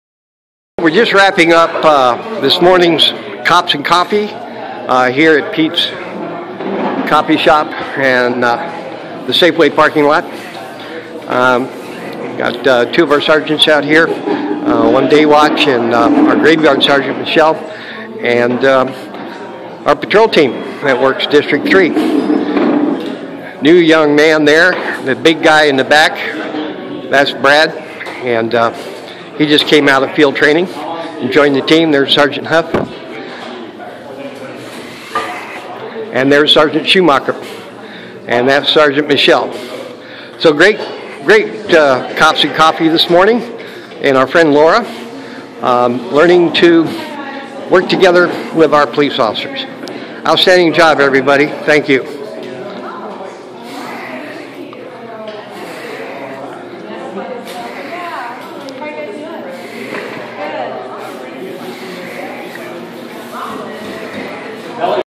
Cops Coffee at Safeway
Crimewatch kicks around community concerns and possible solutions at Safeway with District 3 grave and day watch officers. Would resumption of Community Patrol help? We think it did once before